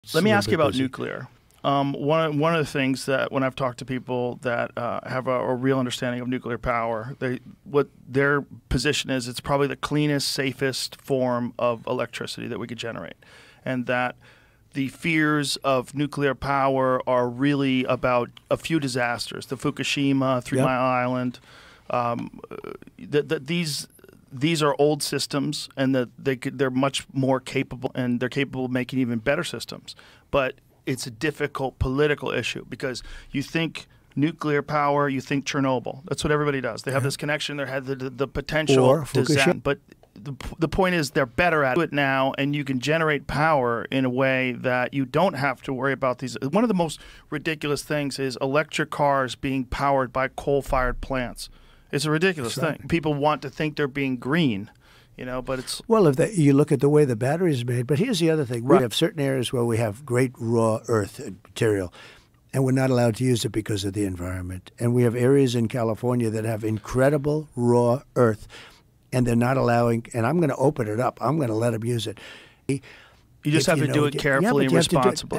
Discover the Future of Energy: Is Nuclear Power the Answer?
Donald Trump, perspectives on nuclear energy's 💡